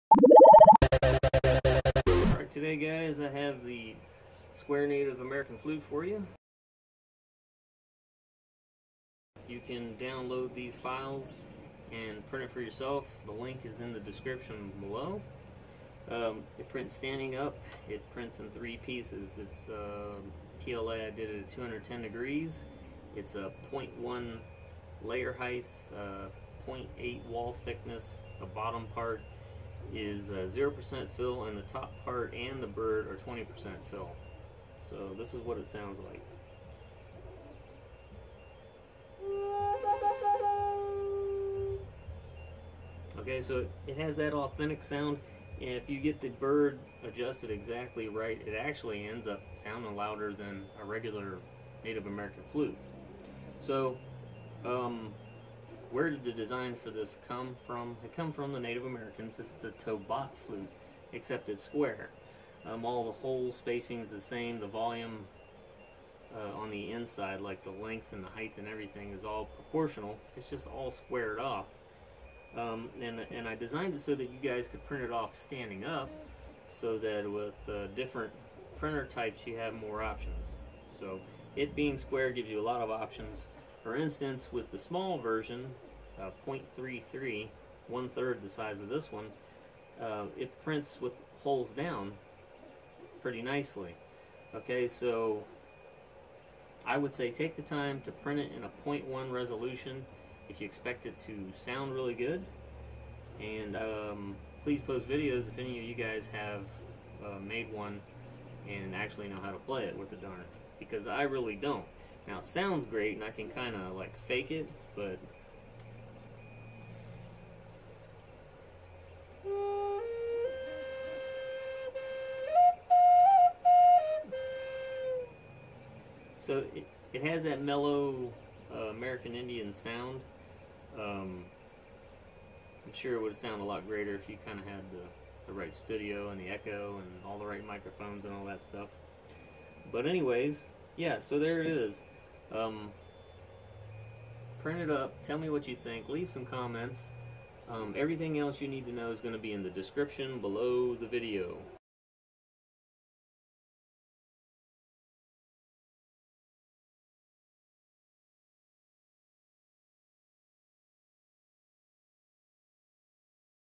Square Native American Flute -3D Print
THIS MUST BE PRINTED IN .1 LAYER HEIGHT OR THINNER!
I designed this flute on Sketchup, use this link to download a copy for yourself:

This was printed in red Hatchbox PLA plastic on a CR-10 HicTop printer.
210 Celsius for the PLA
0.1 layer height
0.4 nozzle
60 Celsius for the Bed Temp
.8 for walls, top, bottom thickness
Use Brim for bed adhesion
Print Flute standing tall
Print with 20% fill for top half and bird but 0% for bottom two pieces.